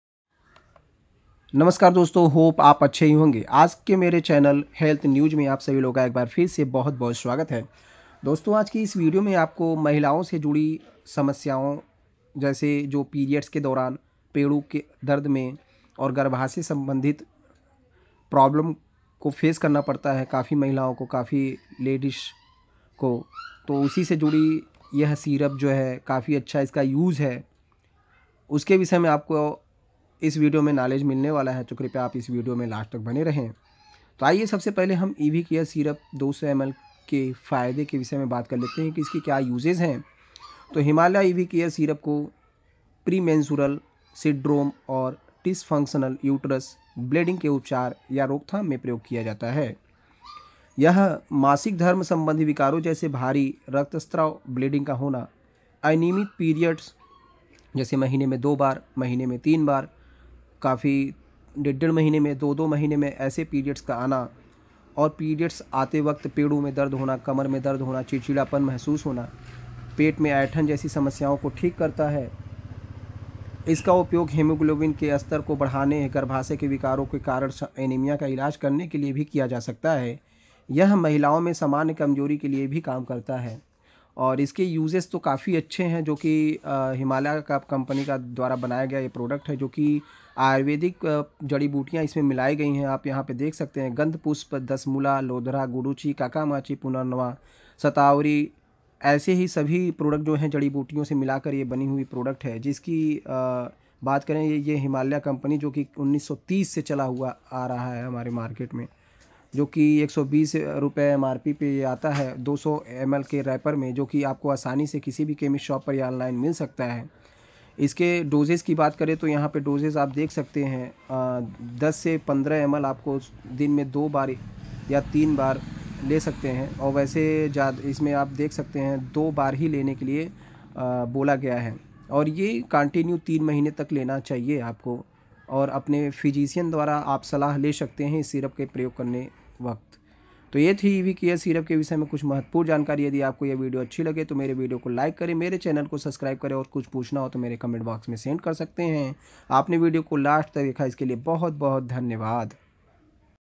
[क्या आपका मासिक धर्म चक्र नियंत्रित नहीं है ! या मासिक धर्म के दौरान दर्द होता हैं।] Evecare Syrup
Second Channel Information about Himalaya Evecare Syrup
Himalaya Evecare Syrup is indicated in the treatment of premenstrual syndrome and dysfunctional uterine bleeding. It helps treat menstrual disorders like heavy bleeding, irregular periods and abdominal cramps. It can be consumed for increasing the level of hemoglobin, regulating reproductive function and also treating anemia due to uterine disorders. It is also recommended for general weakness in women.

Key Ingredients:
Ashoka Tree (Ashoka)
Asparagus (Shatavari)
Lodh Tree (Lodhra)
Malabar Nut (Vasaka)

Key Benefits:
Ashoka Tree comes with estrogenic properties which heal the inner lining of the uterus during menstruation
Asparagus balances the hormonal level and regulates the menstrual cycle
Lodh Tree may improve fertility by regulating ovarian hormones
Malabar Nut has anti-inflammatory and analgesic properties that provide relief from pain during dysmenorrhea (menstrual cramps)

Directions For Use:
Use as directed by the physician.

Safety Information:
Store in a cool dry place away from direct sunlight
Keep out of reach of the children
Use under medical supervision